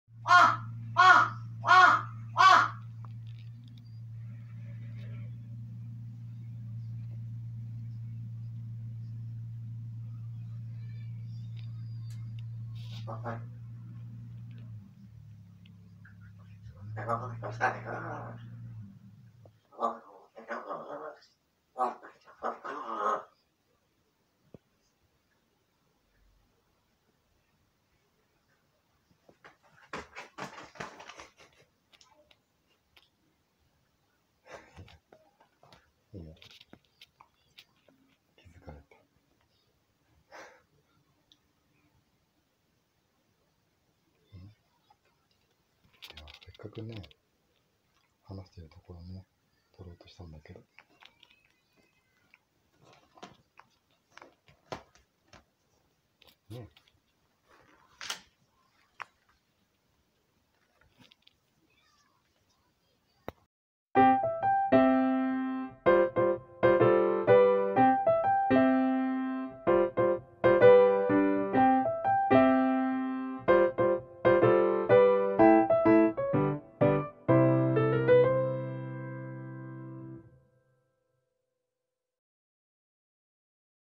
喋るカラスの姿
初めて独り言を言ってる姿を撮影できました。
喉がモニョモニョ動いて窓の外に話しています。